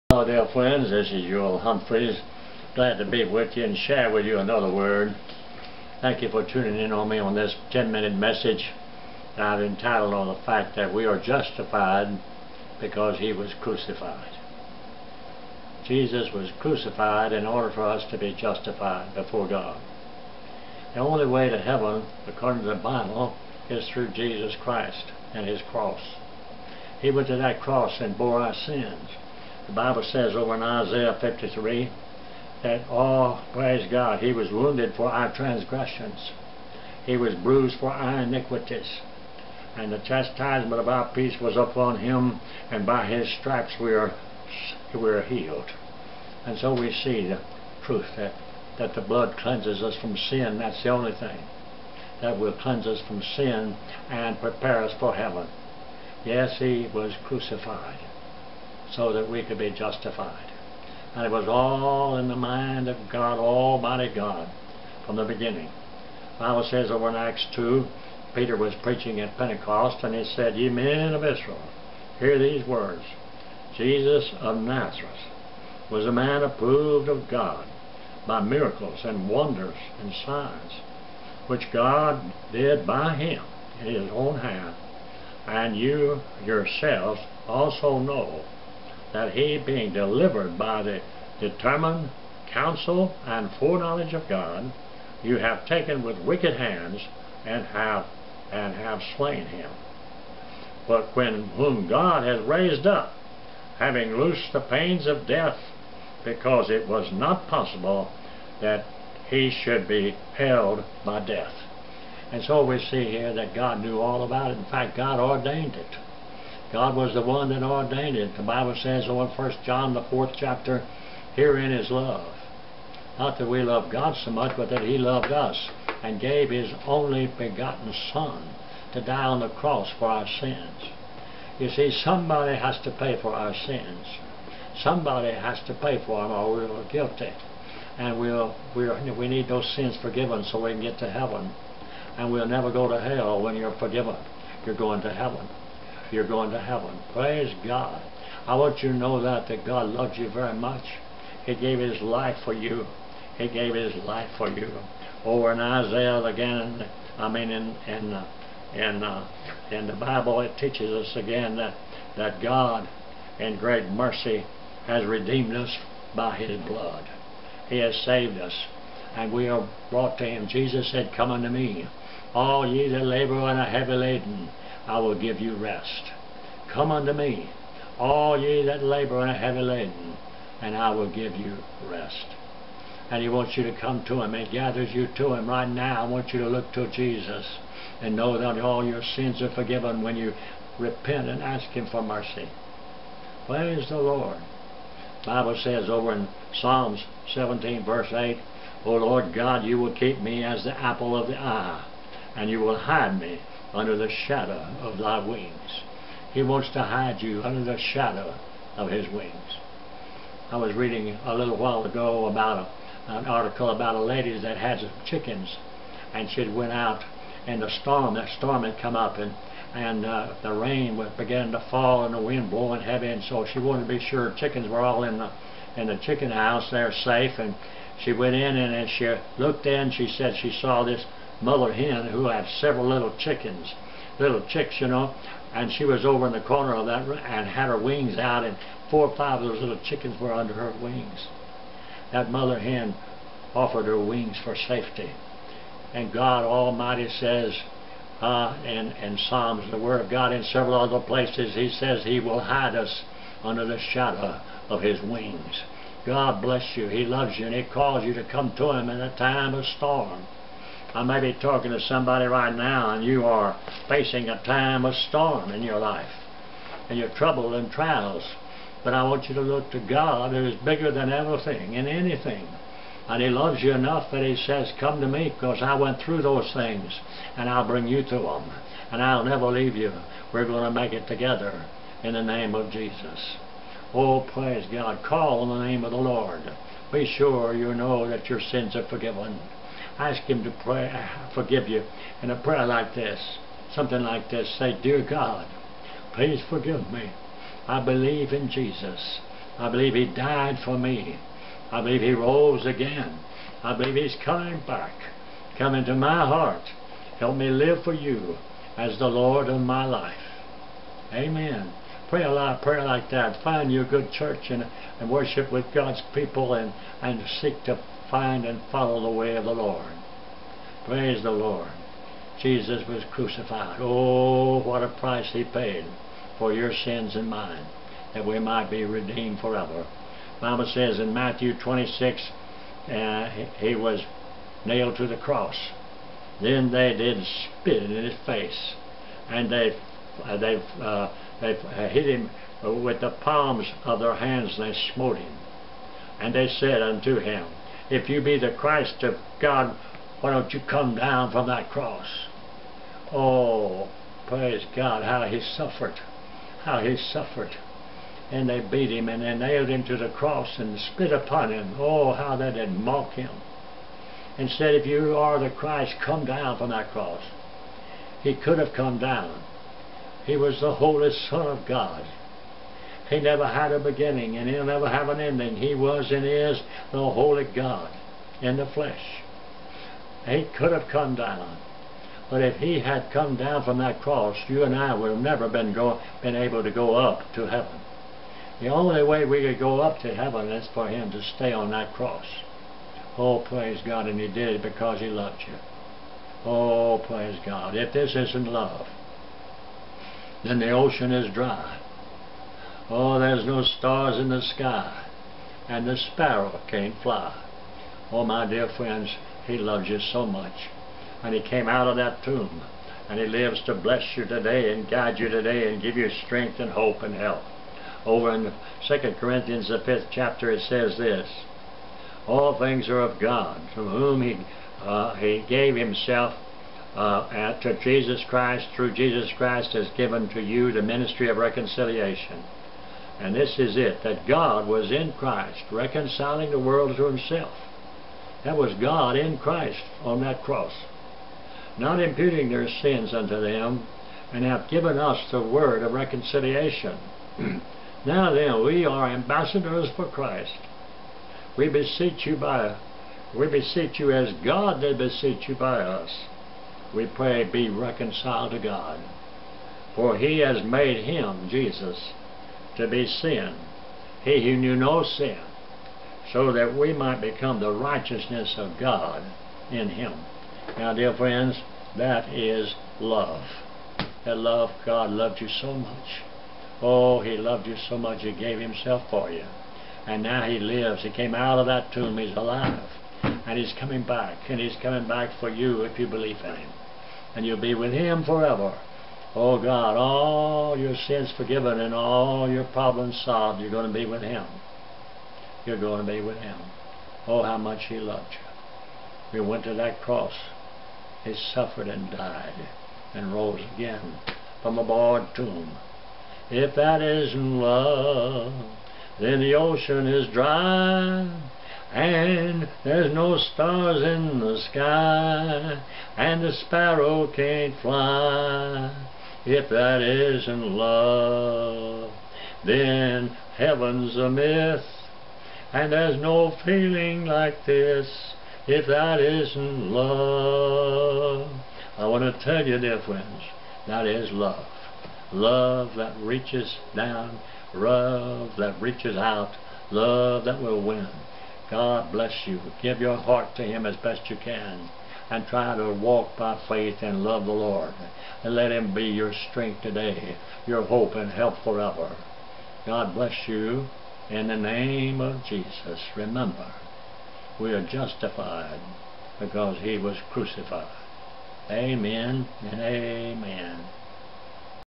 Bible Reflections He was crucified so we could be justified
Bro. Humphreys speaks on the vutal importance of the crucifiction of Jesus Christ. He usesscriptures to prove this truth.